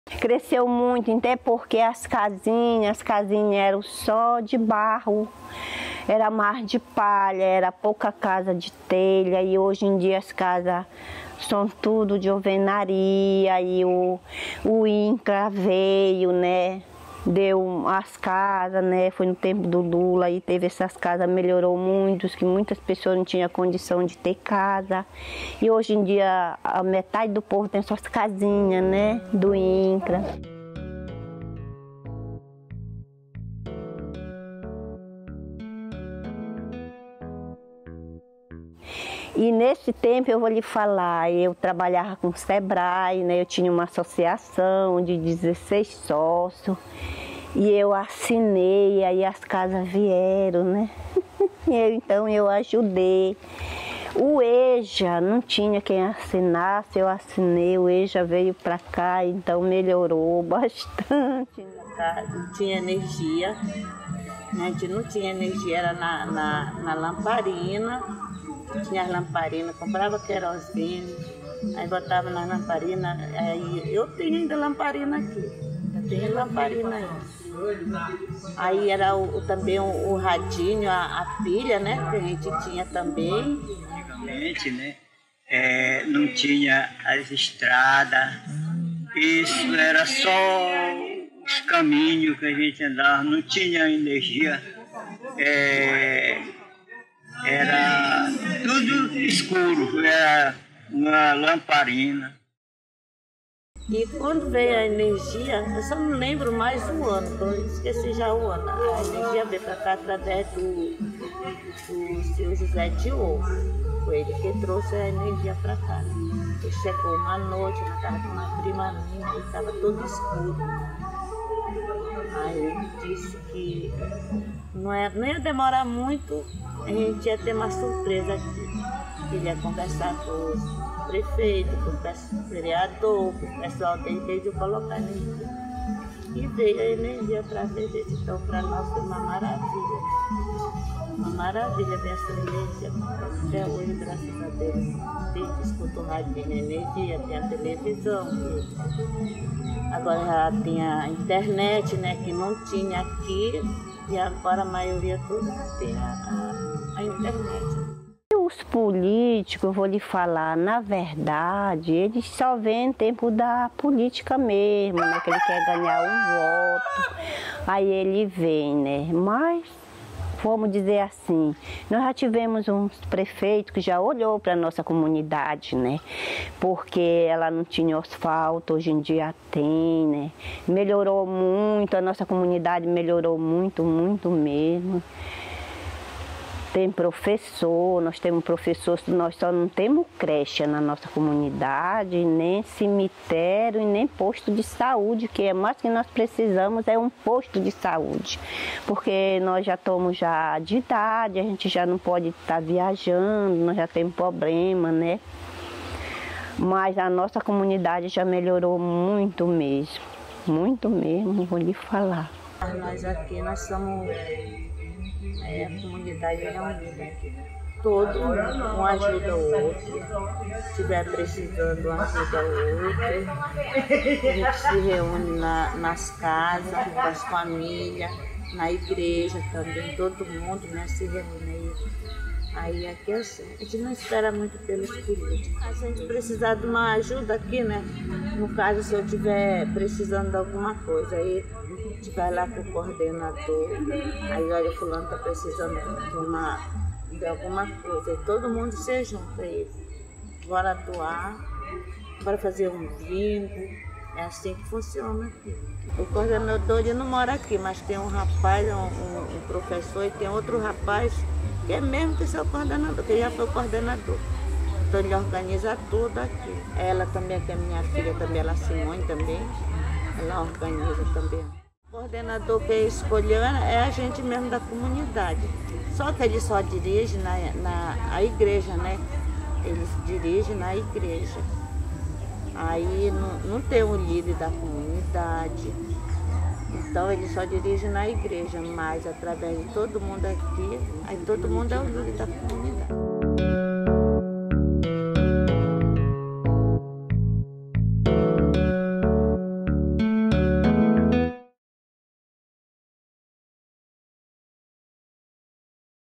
Vila Que Era (Pará) - ESTRUTURA
Entrevista com moradores da Vila Que Era, no estado do Pará (Brasil), sobre como a estrutura física da comunidade foi evoluindo com o passar do tempo. Este vídeo foi realizado durante as atividades do Wiki Ocupa Bragança, organizado em agosto de 2023 pelo Wiki Movimento Brasil.